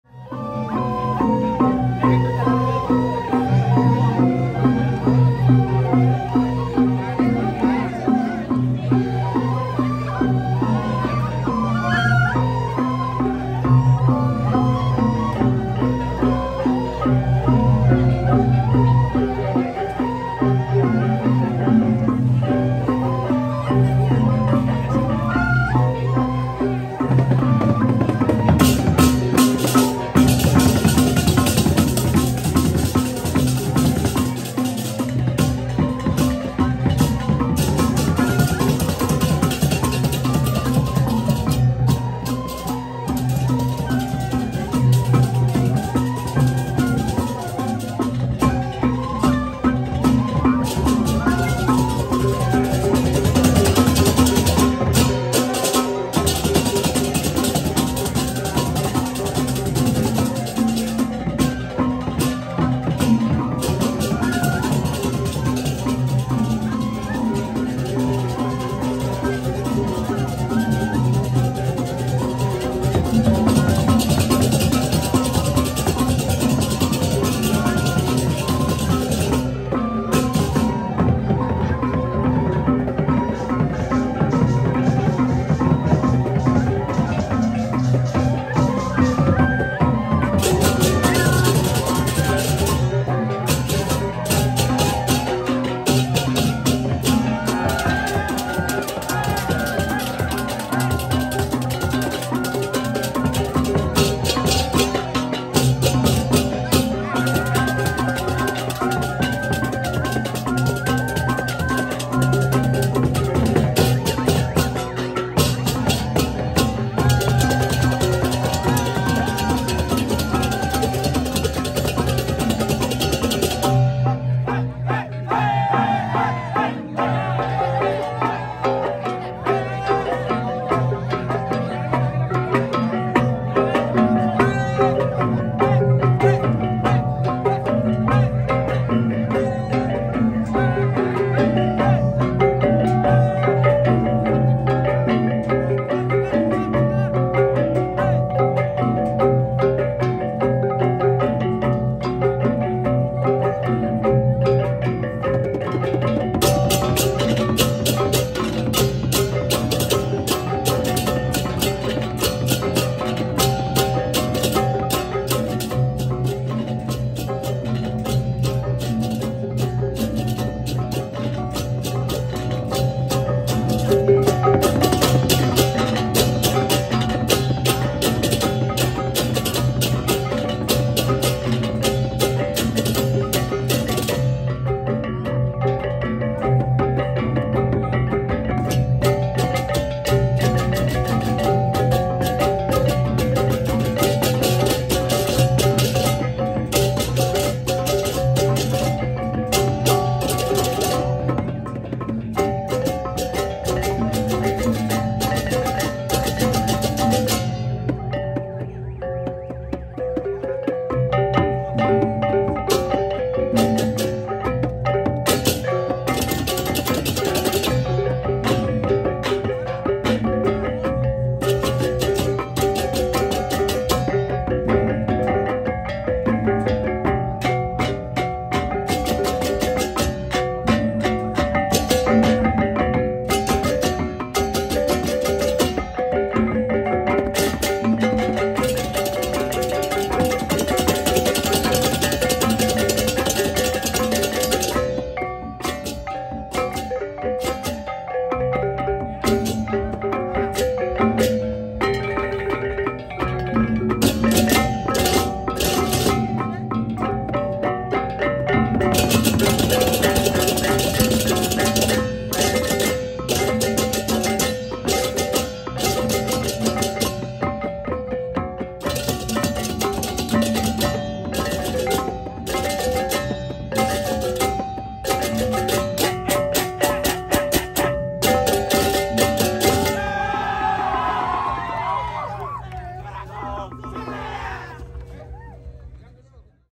Baleganjur Suling Cantik Banjar Kebon | Parade Gebogan & Baleganjur Tanah Lot Festival 2023
Tanah Lot art & food Festival IV
dari tanggal 22 - 25 Juni 2023